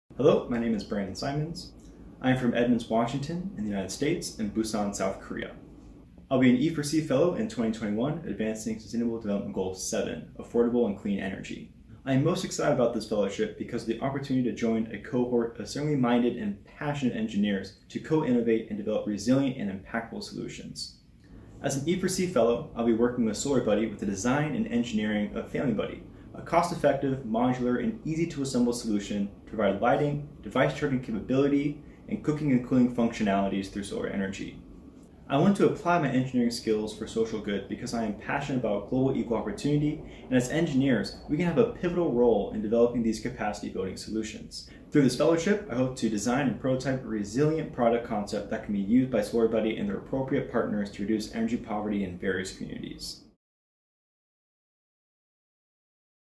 Brandon Simons - E4C 2021 Fellow
Brandon Simons, one of our 2021 Fellows, introduces himself, what excites him about the Fellowship and the impact he hopes to have through the Fellowship.